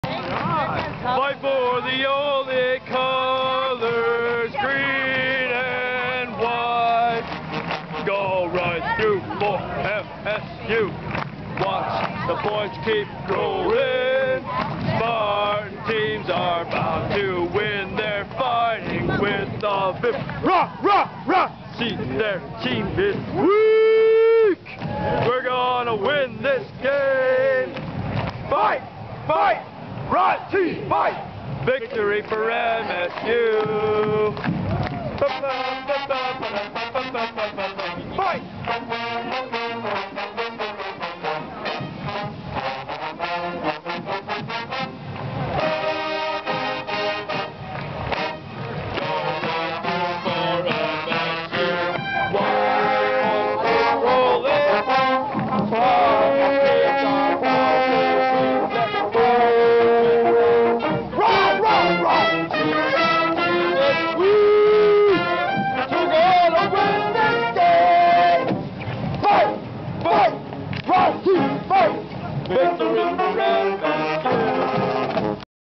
MSU ND 2007 Pregame
Greatest Fight Song in all of the land!  Pregame ND and Michigan State University